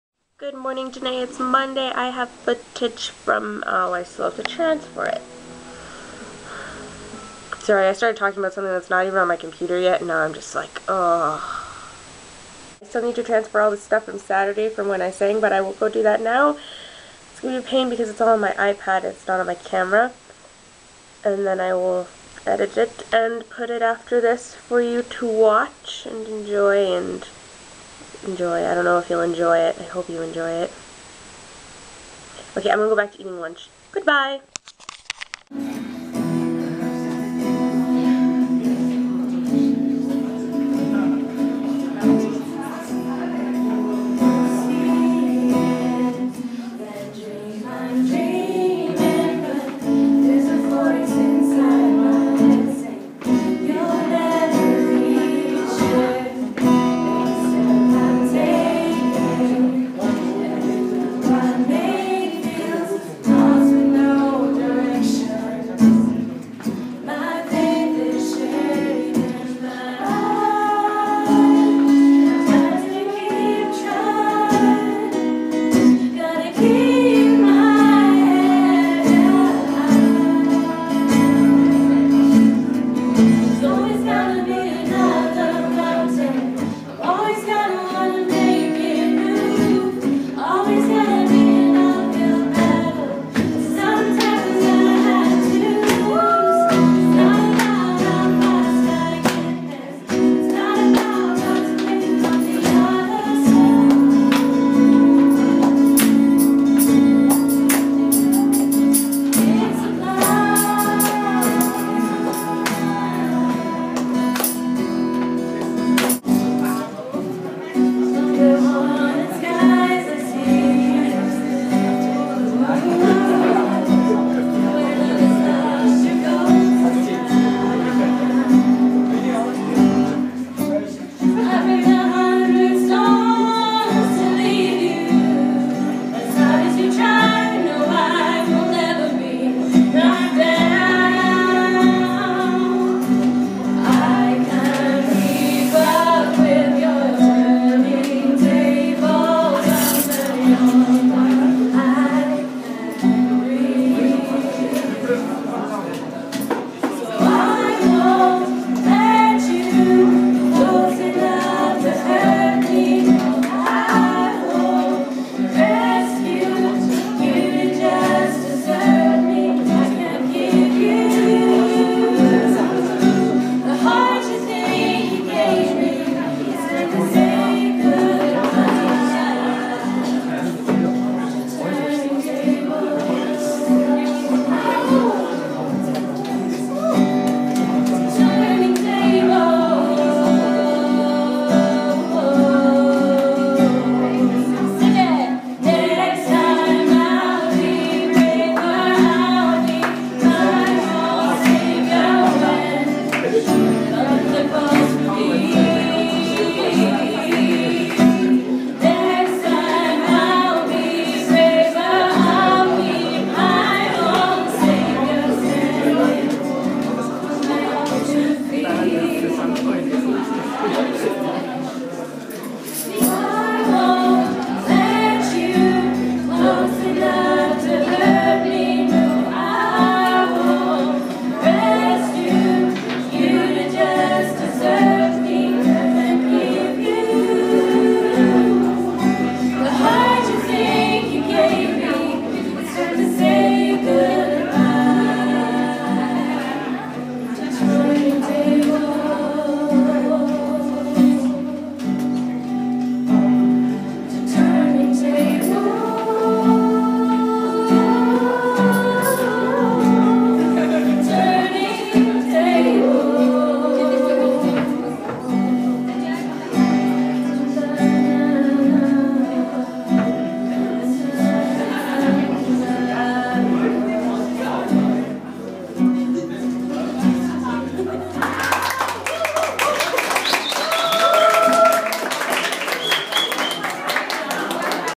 Open Mic
In which Whitney sings at open mic night @ Ubean.